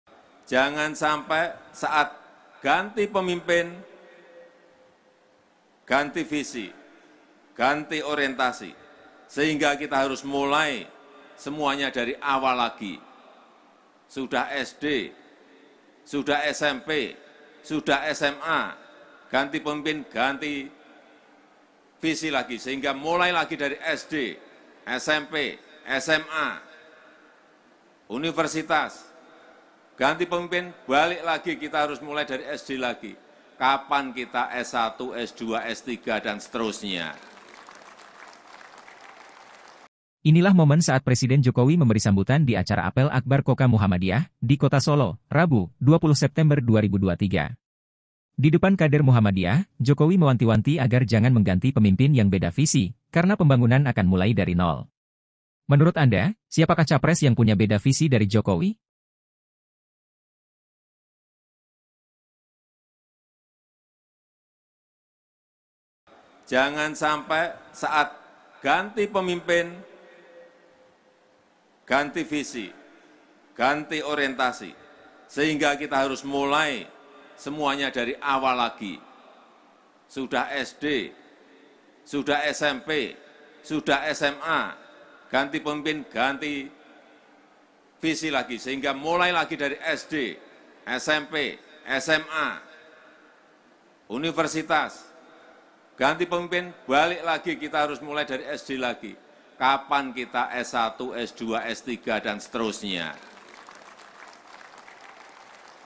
Jokowi: Jangan Ganti Pemimpin yang Beda Visi! Adakah Capres yang Dimaksud Jokowi?
Inilah momen saat Presiden Jokowi memberi sambutan di acara Apel Akbar Kokam Muhammadiyah, di Kota Solo, Rabu (20/9/2023).

Di depan kader Muhammadiyah, Jokowi mewanti-wanti agar jangan mengganti pemimpin yang beda visi, karena pembangunan akan mulai dari nol.

Menurut anda, siapakah Capres yang punya beda visi dari Jokowi?